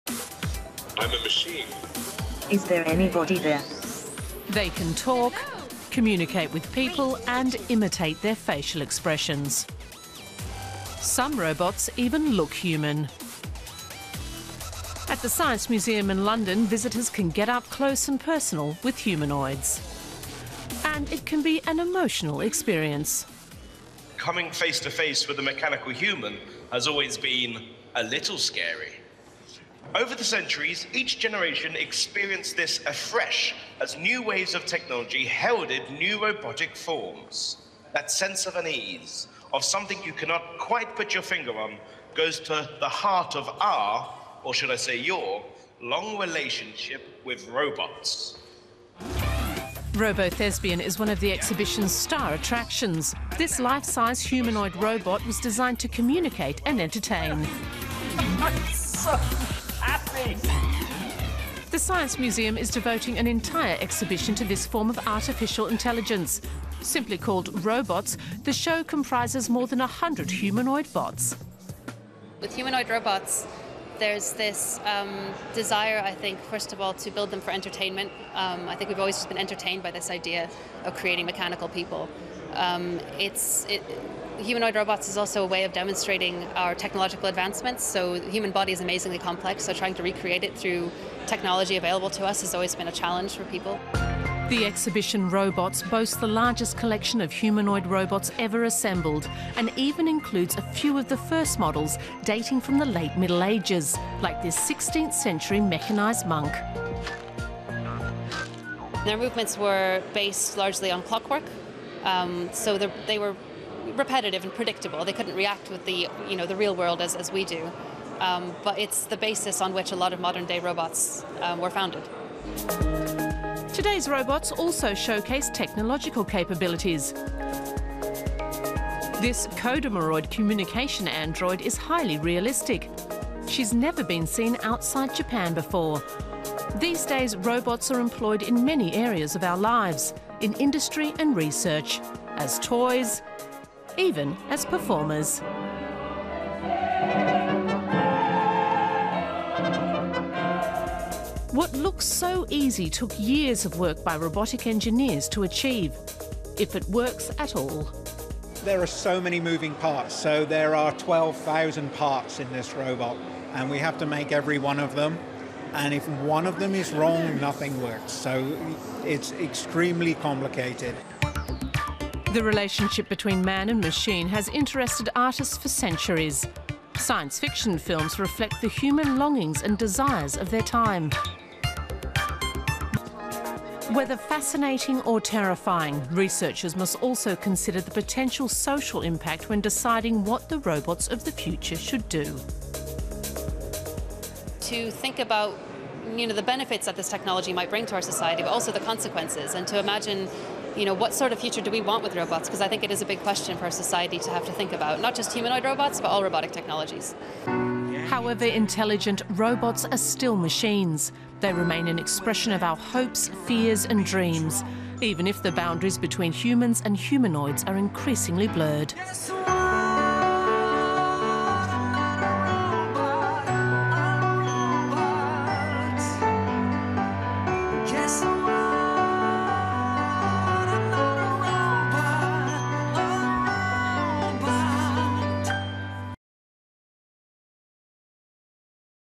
The 500 year history of humanoid robots | Euromaxx
An exhibition at the Science Museum in London is the biggest gathering to date of robots made to resemble humans in form and function. More than a hundred embody five centuries of amazing R&amp;D.